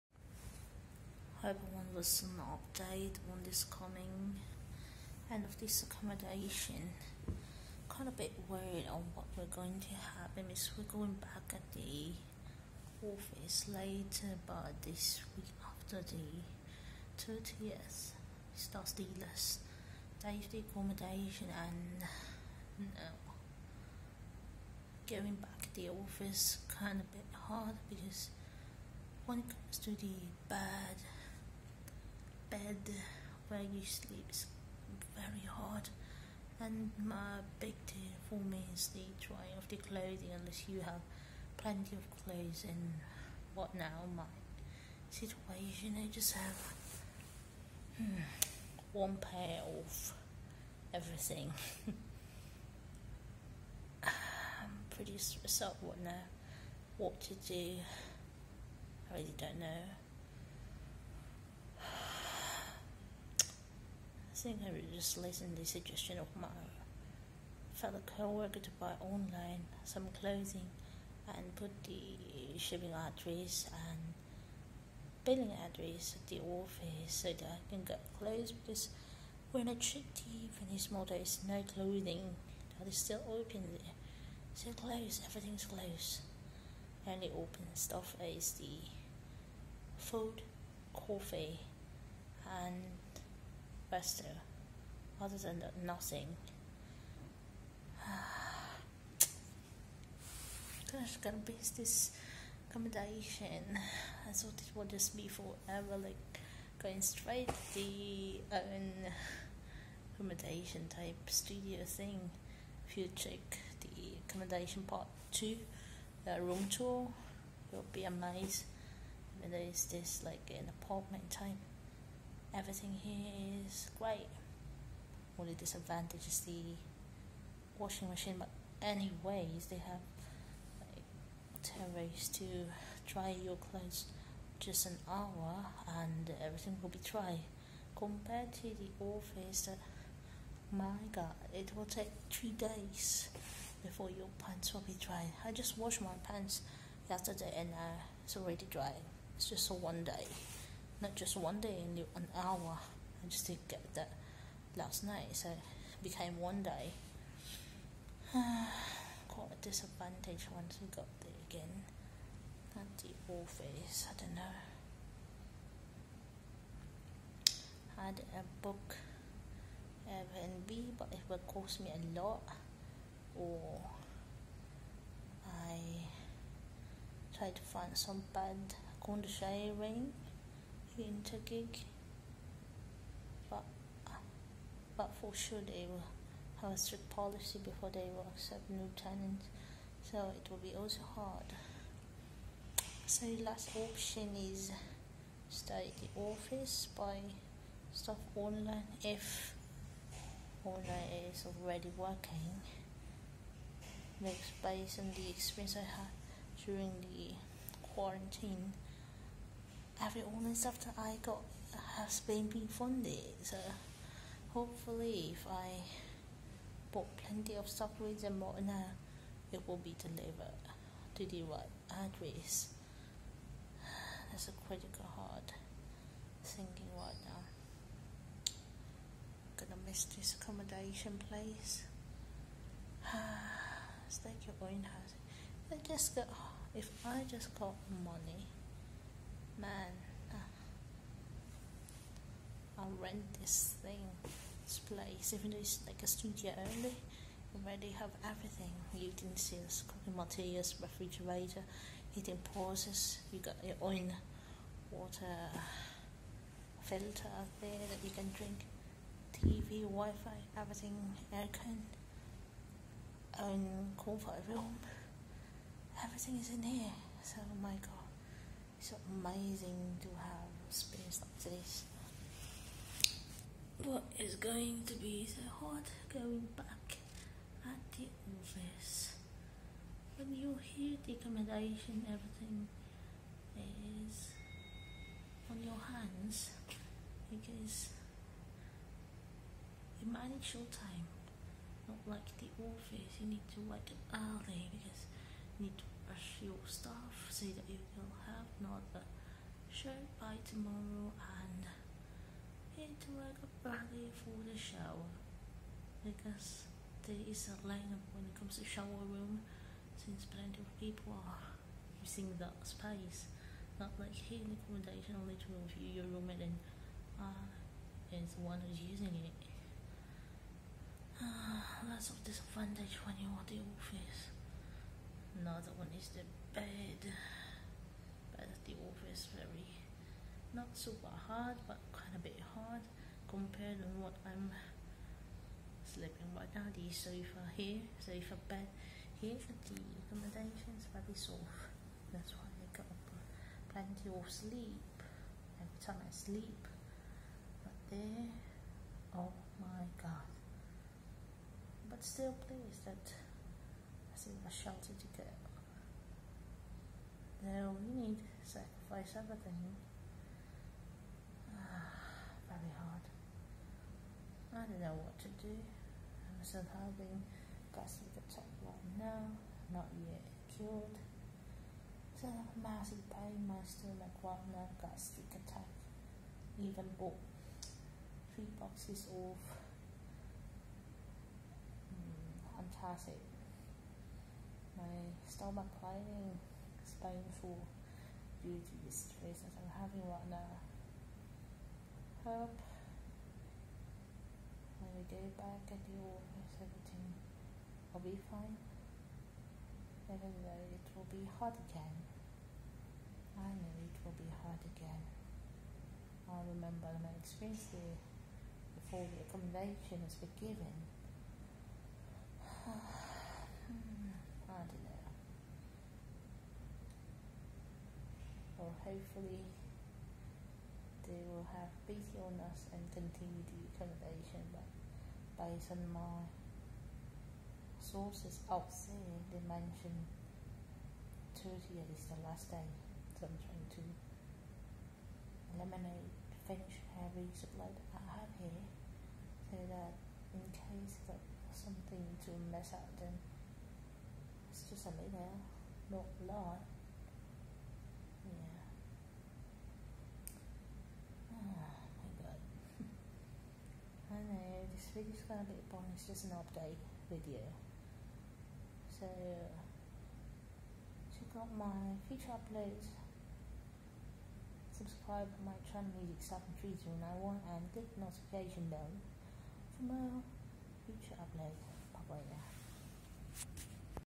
Update Video May 26,2020
This is the update on what might happen with us here in the accommodation after on the 30th.  Hope you still like it.  Comment now here in my youtube channel "music73091".